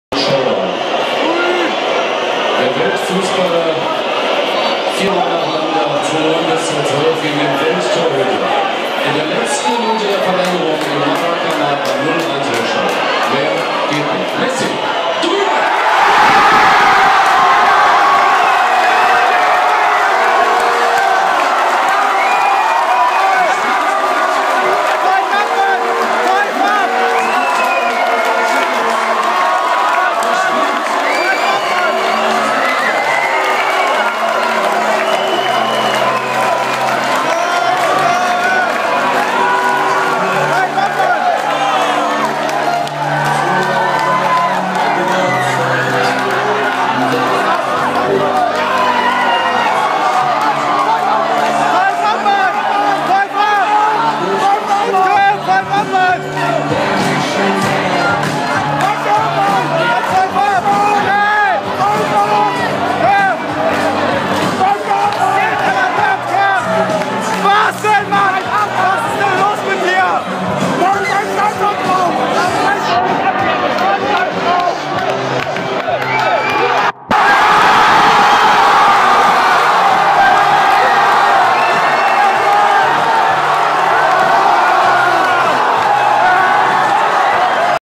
Deutschland ist WELTMEISTER 2014
Die letzten Sekunden des WM-Finale 2014 zwischen Deutschland und Argentinien (1:0n.V.)